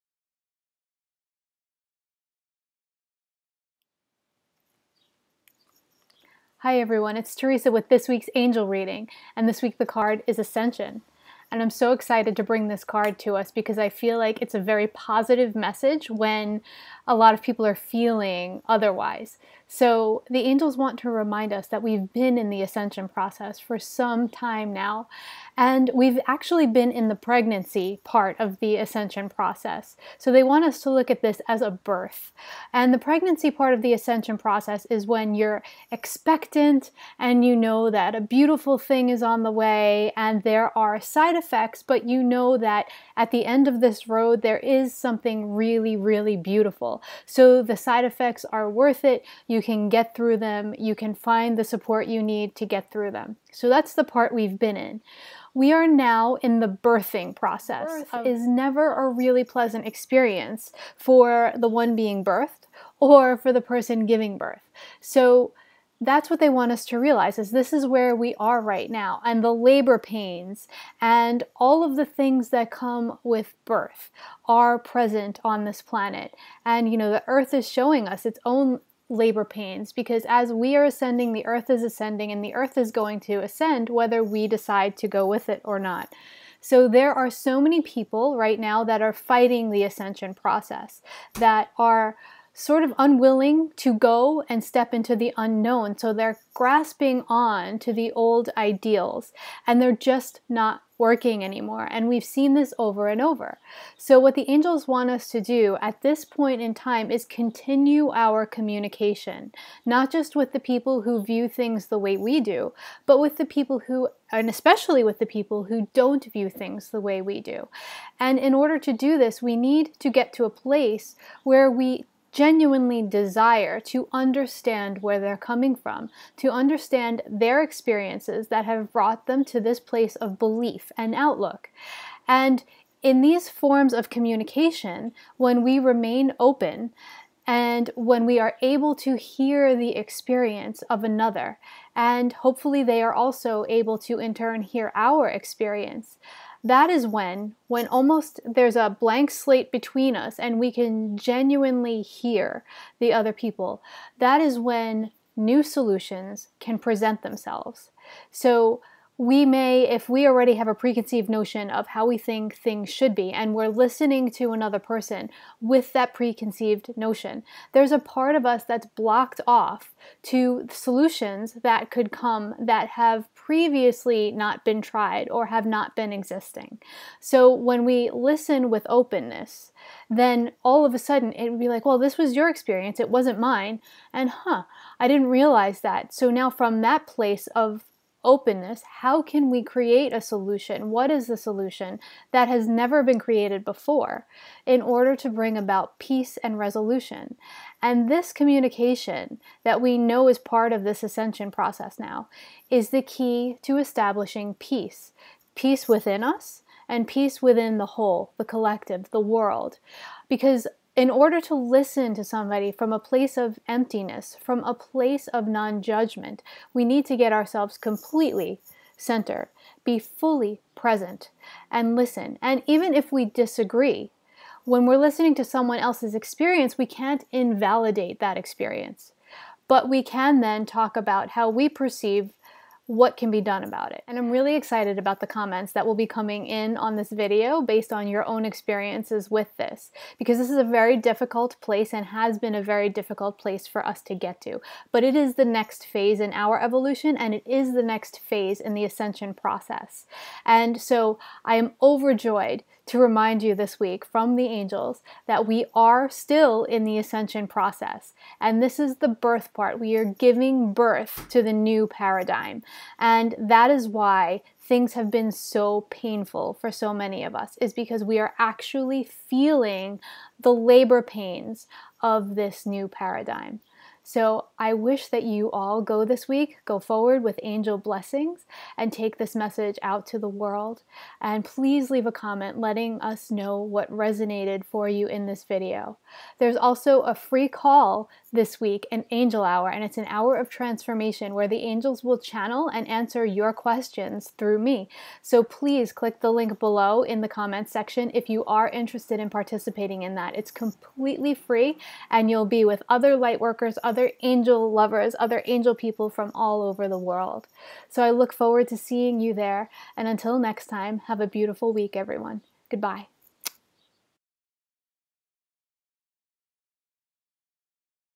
Angel Messages with Theresa: 7.9.17
ANGELIC MESSAGES FOR SPIRITUAL GANGSTAS

BIRTHING ASCENSION

This week, the Angels are talking about ASCENSION and reminding us that we are still in the midst of this process. This week, the Angels share:
• The key to getting us from ascension to peace.
• Where we are in the process and why it feels different than it used to.
• How openness can lead to solutions.

LET US KNOW! What resonated for you in this week’s reading?

If you would like to join in on the Angel Transformation Call this Thursday, click the link below for more information and reserve your spot!